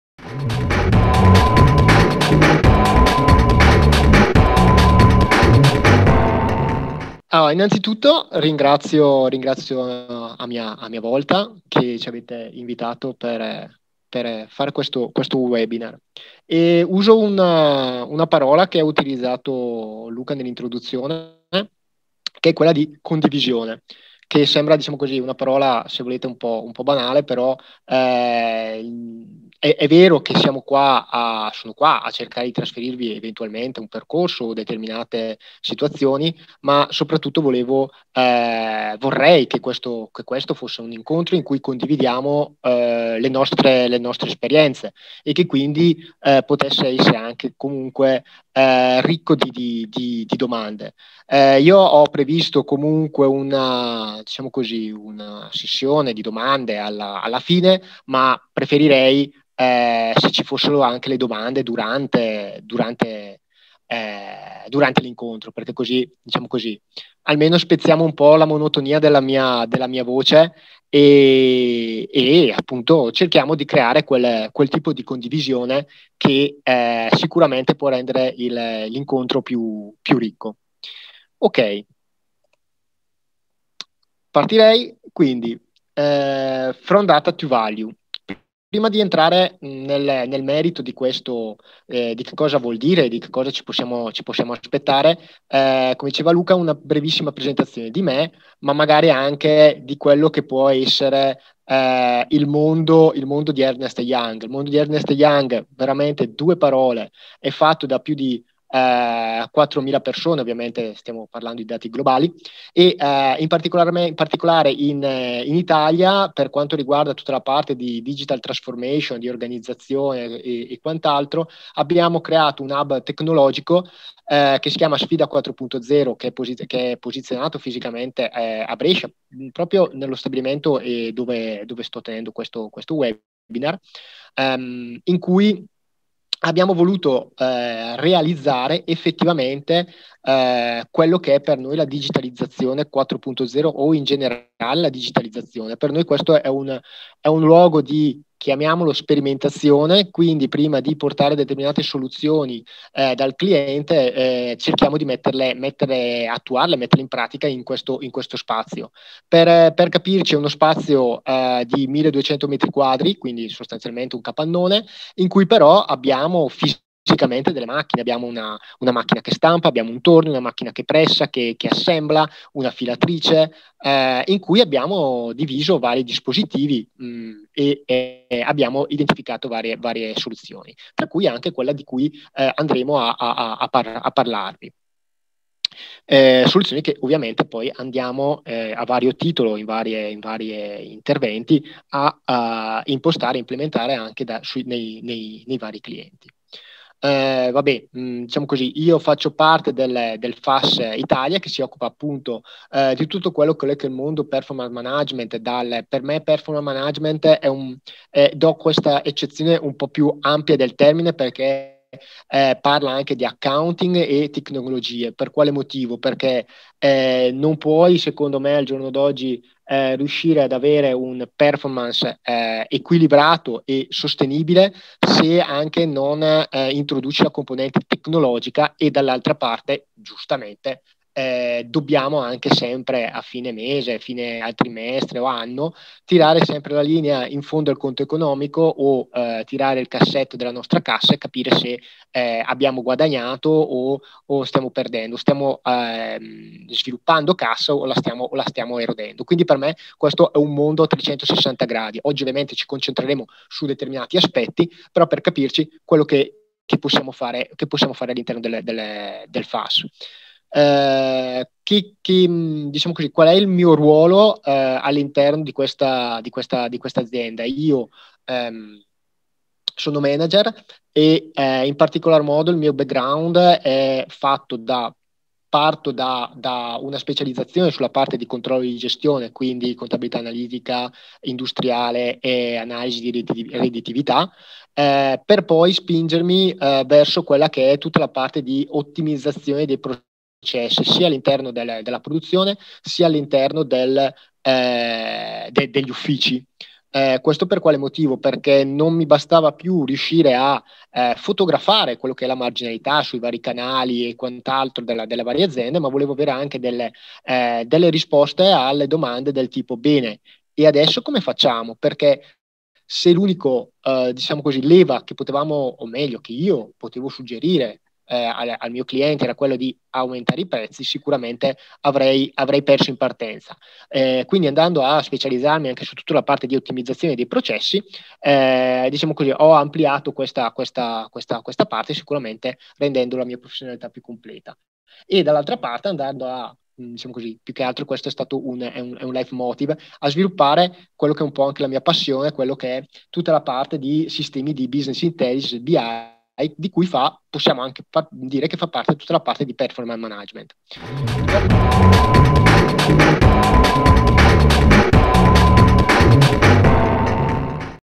From Data to Value - 1
Alessandro Franzoni e Sara De Berardinis,  professionisti esperti di sistemi di controllo di gestione, misurazione ed ottimizzazione delle performance aziendali, reporting e di digital transformation offrono un’ampia panoramica sulla conversione di dati in valori nelle attività data-based.